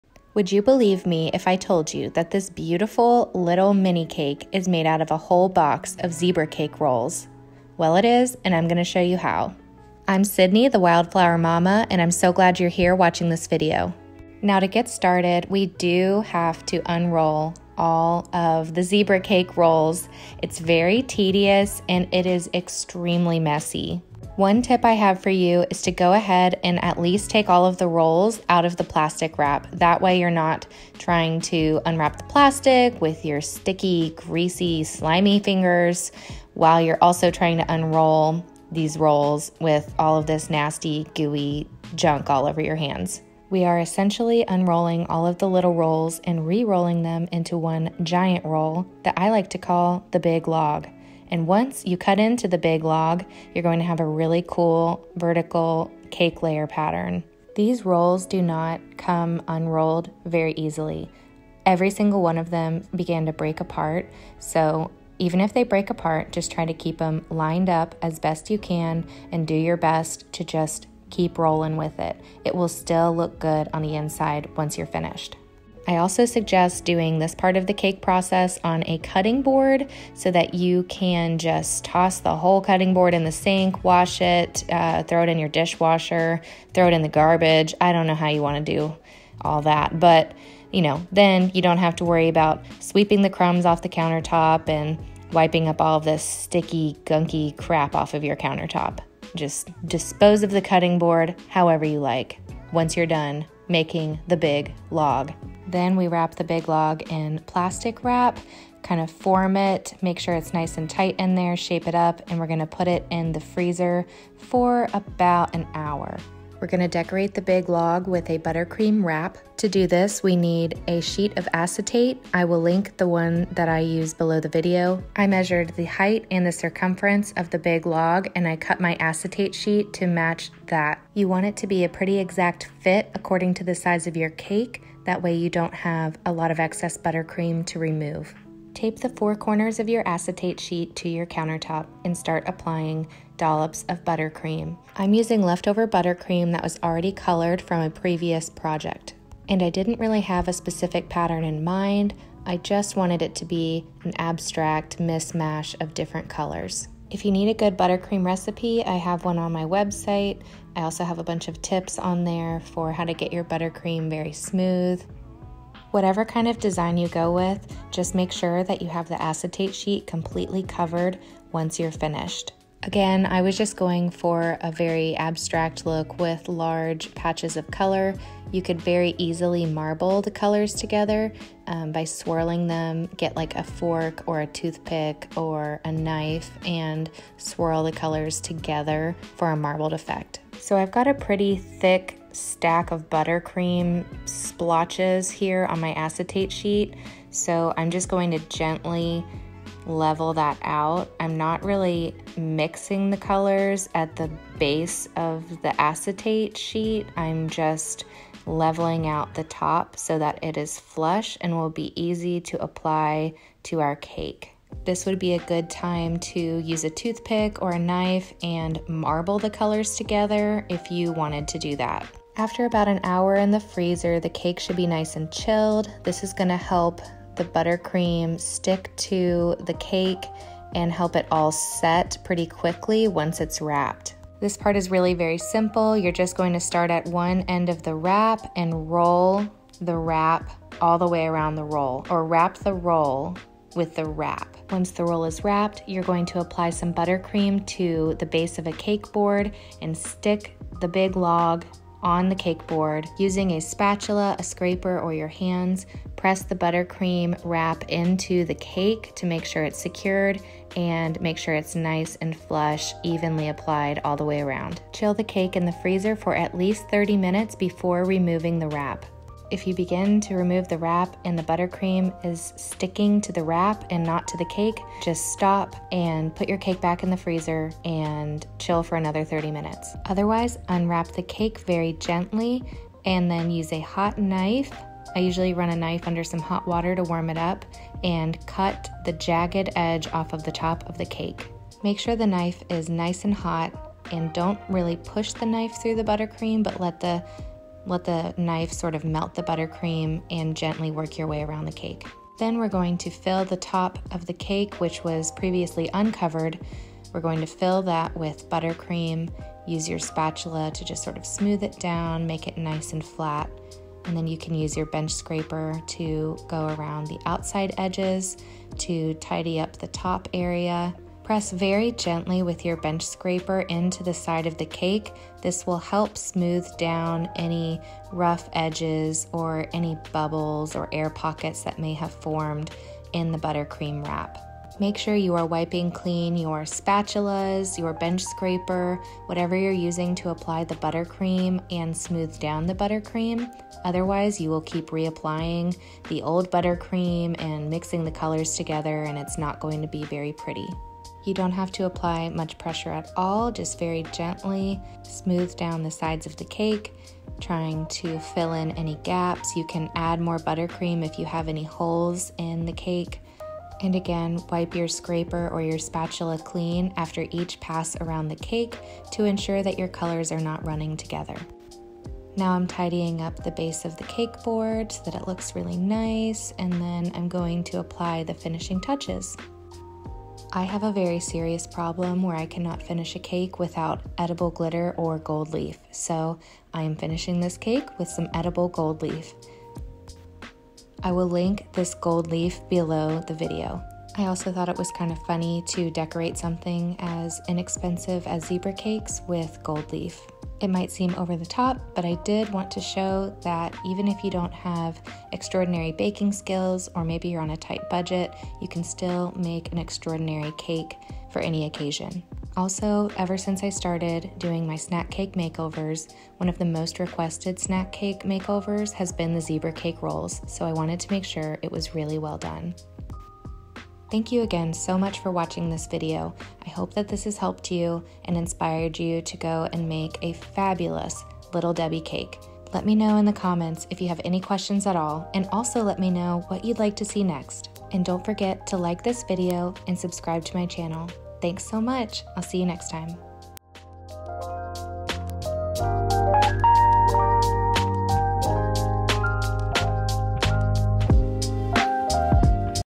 Make a Stunning Cake out of Zebra Cake Rolls
Here’s the super easy cake decorating tutorial on how I transformed a box of Zebra Cake Rolls into a beautiful mini cake using a buttercream wrap!

Links mentioned in the video:

Acetate Sheets

Gold Leaf